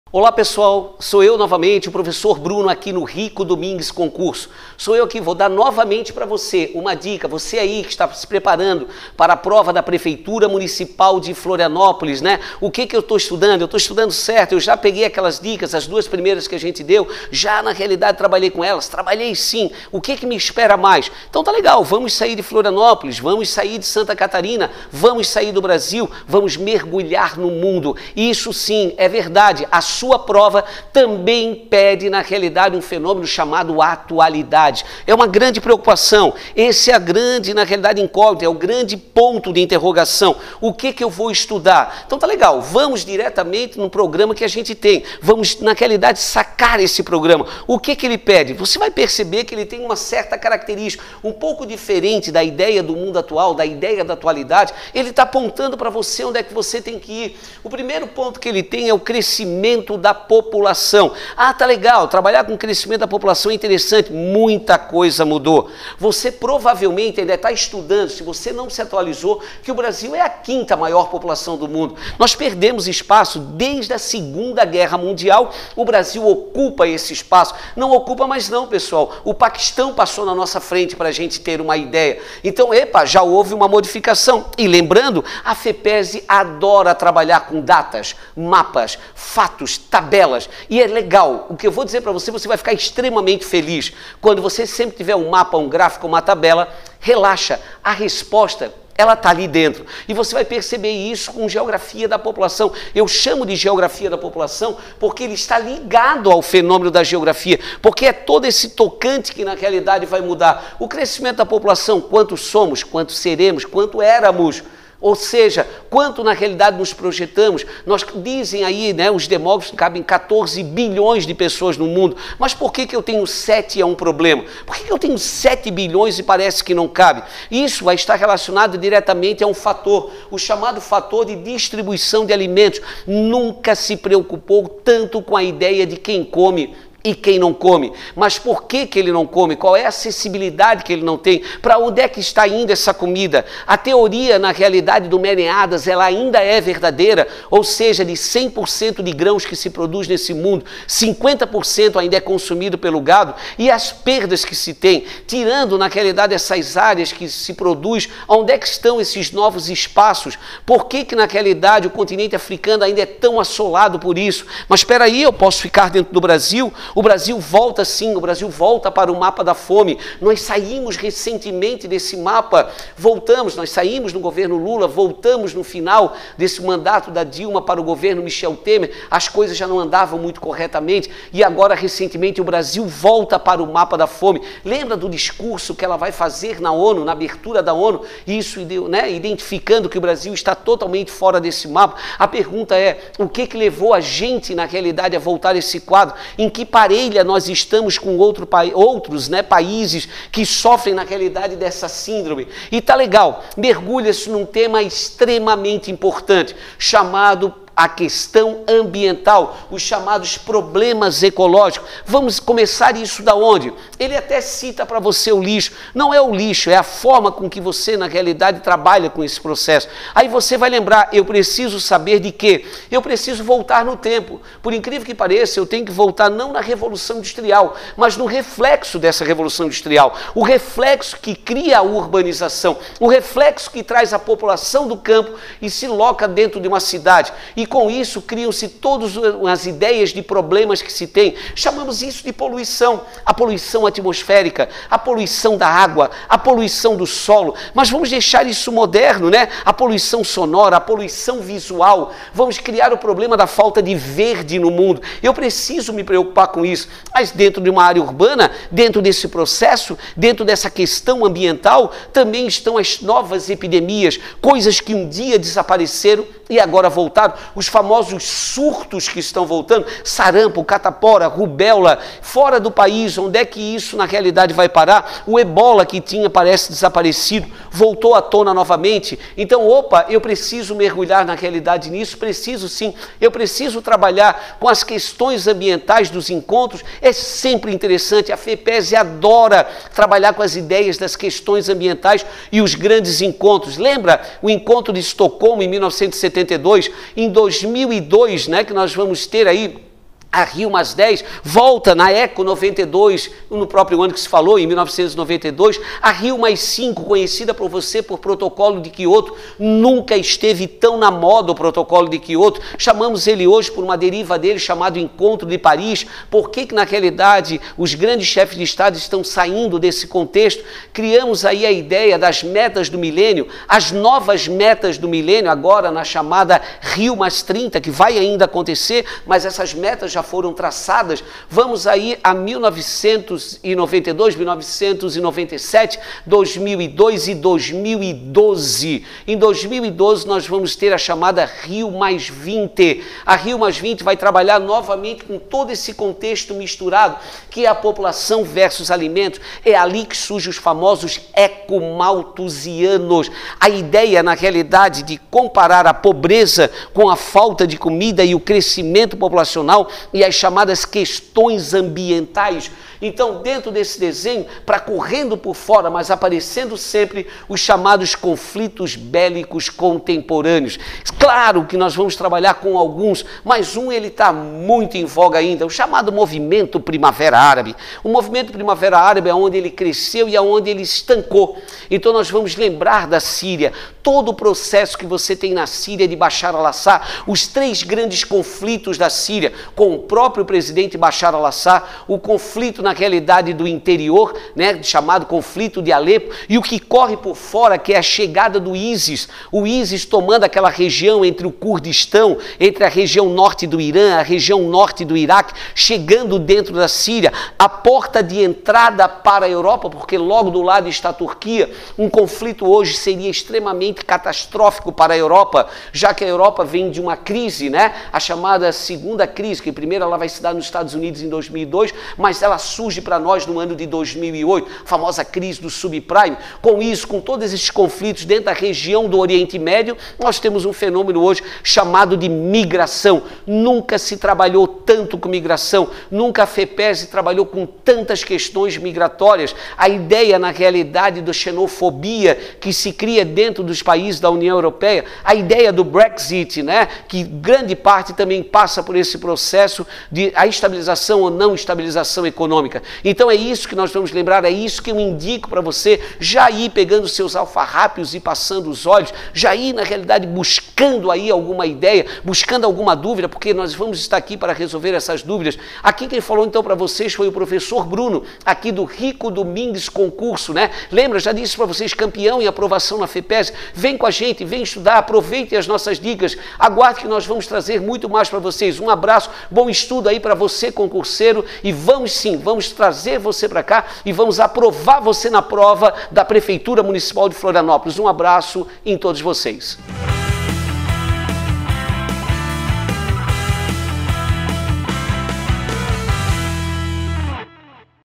Concurso Prefeitura de Florianópolis: Mundo atual | Atualidades
Nesse vídeo, o professor de Atualidades do Rico Domingues, Luiz Carlos Bruno, fala sobre o mundo atual, para você se preparar para o Concurso Prefeitura de Florianópolis!